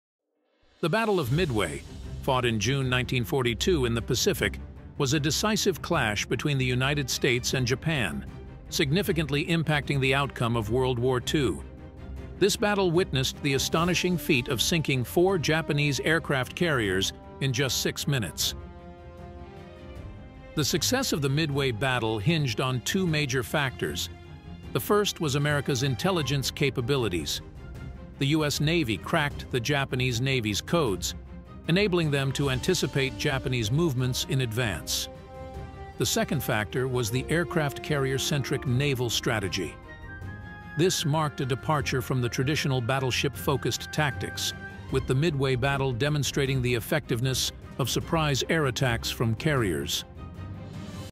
(1 of 2) "The Battle of Midway: A Pivotal Moment in the Pacific War"
"The Battle of Midway, fought in June 1942 in the Pacific, was a decisive clash between the United States and Japan, significantly impacting the outcome of World War II. This battle witnessed the astonishing feat of sinking four Japanese aircraft carriers in just six minutes.

The success of the Midway battle hinged on two major factors. The first was America's intelligence capabilities. The U.S. Navy cracked the Japanese Navy's codes, enabling them to anticipate Japanese movements in advance. The second factor was the aircraft carrier-centric naval strategy. This marked a departure from the traditional battleship-focused tactics, with the Midway battle demonstrating the effectiveness of surprise air attacks from carriers.